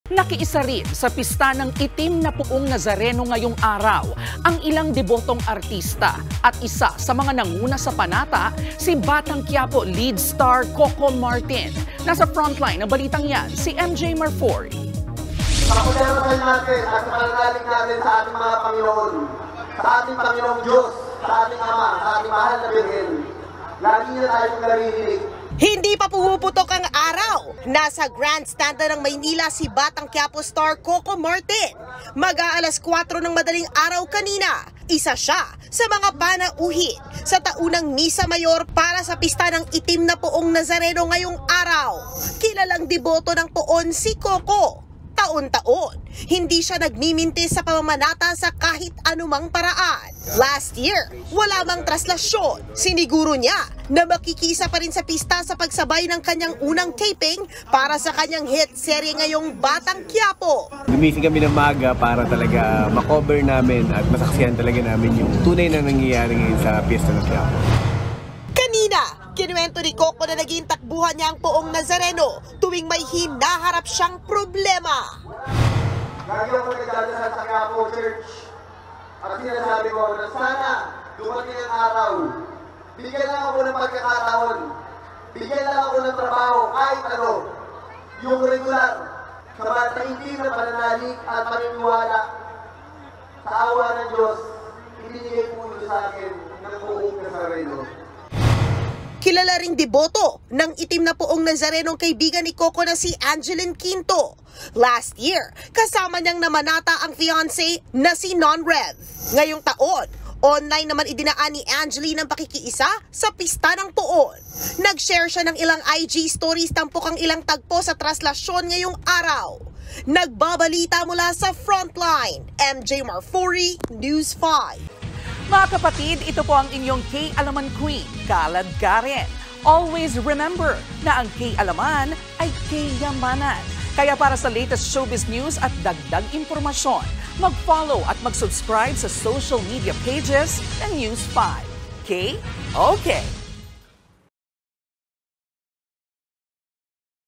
Coco Martin, nakiisa rin sa pista ng Poong Itim na Nazareno | Frontline Pilipinas
FrontlinePilipinas | Nakiisa rin sa pista ng Poong Itim na Nazareno ang ilang debotong artista. Isa sa mga nanguna sa panata si BatangQuiapo lead star CocoMartin. Traslacion2024 News5 | via MJ Marfori